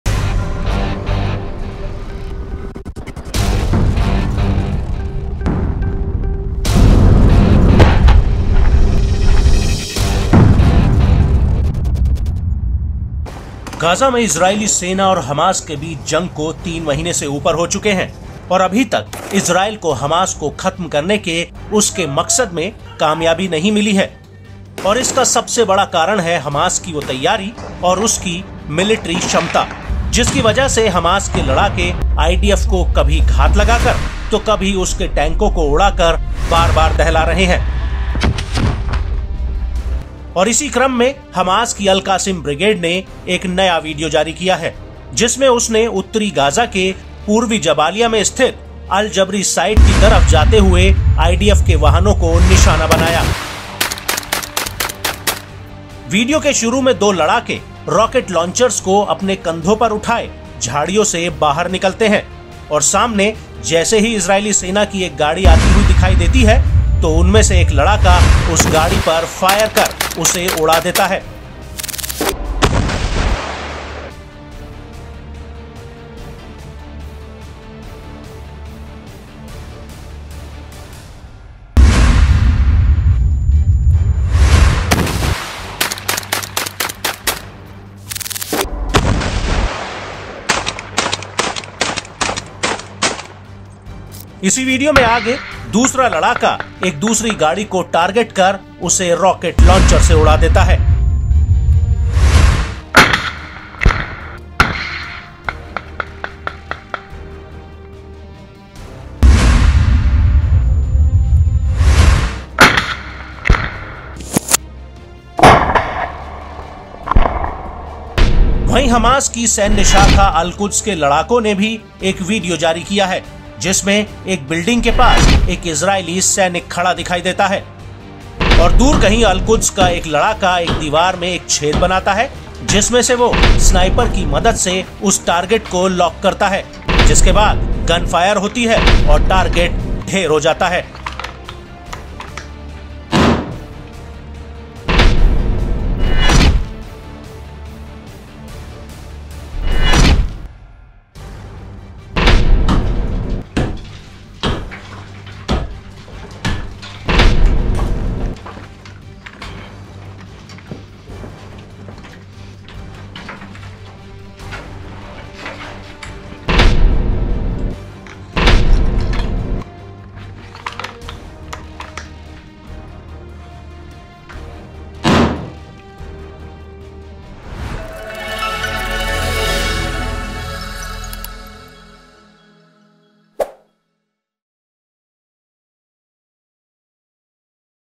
Israel Hamas War Update: Al-Qassim Brigades ने तबाह की IDF की गाड़ियां | Netanyahu | Palestine | Gaza
It has been more than 3 months since the war between the Israeli army and Hamas in Gaza and till now Israel has not succeeded in its objective of eliminating Hamas and the biggest reason for this is the preparation of Hamas and its military capability due to which Hamas fighters are repeatedly terrorizing the IDF, sometimes by ambushing them and sometimes by blowing up its tanks.

गाज़ा में इज़रायली सेना और हमास के बीच जंग को 3 महीने से ऊपर हो चुके हैं और अभी तक इज़रायल को हमास को ख़त्म करने के उसके मकसद में कामयाबी नहीं मिली है और उसका सबसे बड़ा कारण है हमास की तैयारी और उसकी मिल्ट्री क्षमता जिसकी वजह से हमास के लड़ाके IDF को कभी घात लगाकर तो कभी उसके टैंकों को उड़ाकर बार बार दहला रहे हैं